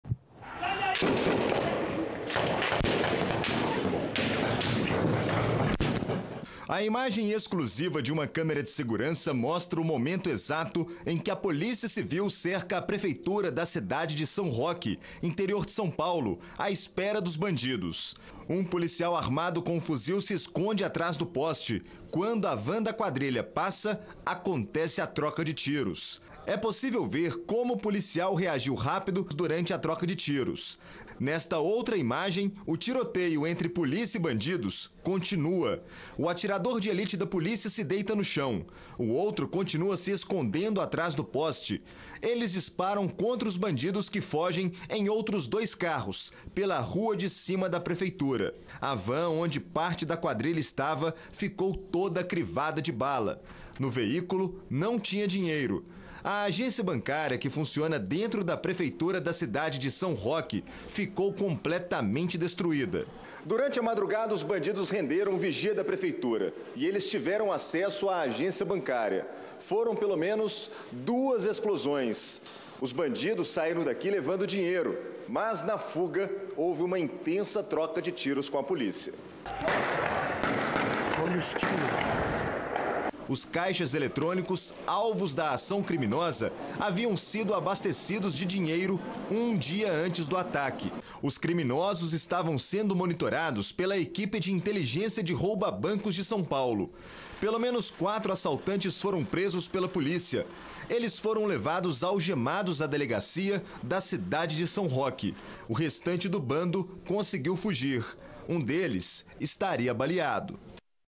Bando explode caixas eletrônicos em São Roque
Explosão, tiros e correria aconteceram dentro da prefeitura.